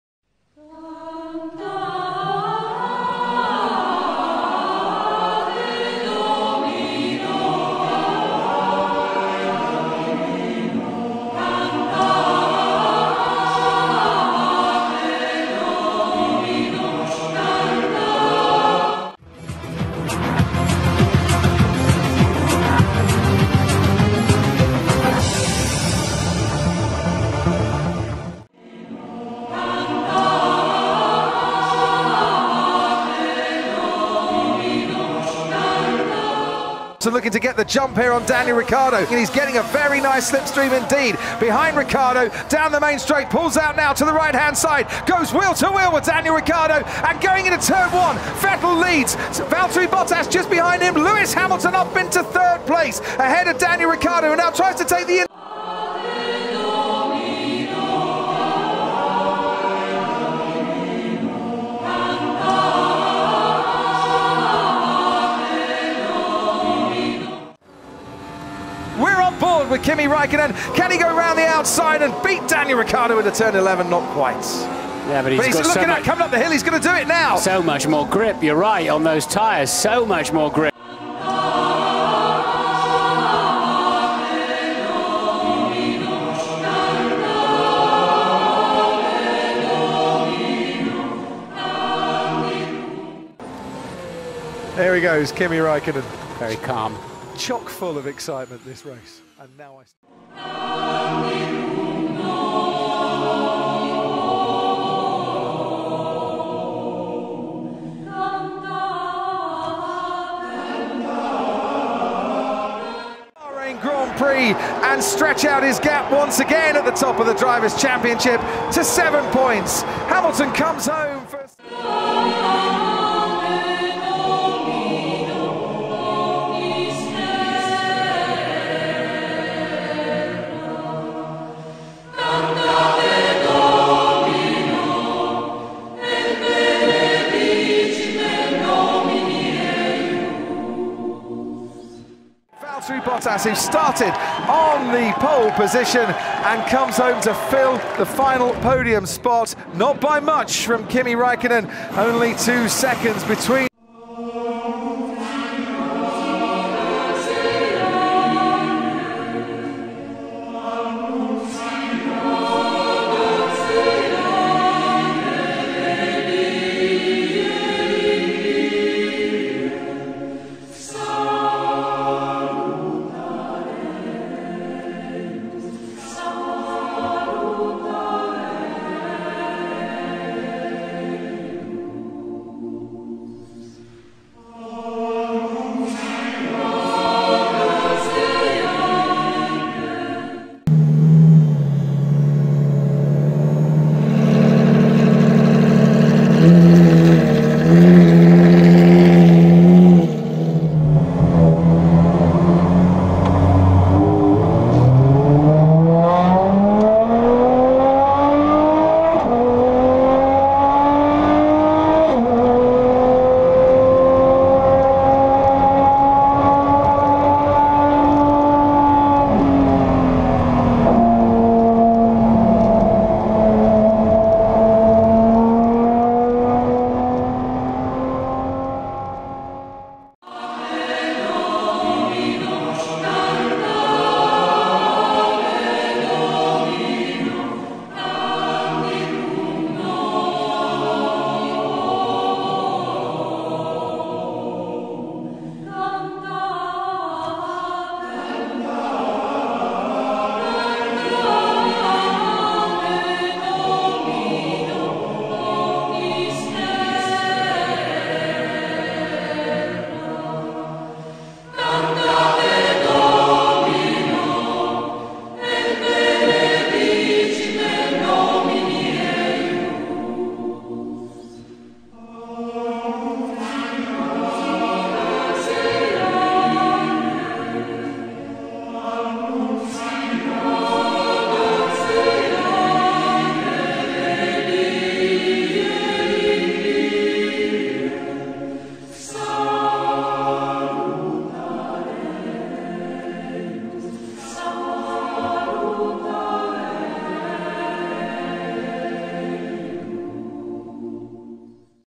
Ferrari NDT
Desarrollo paralelo de NDT Italia y Ferrari. NDT Italia finalmente llega al Continente Americano gracias a NDT Latinoamérica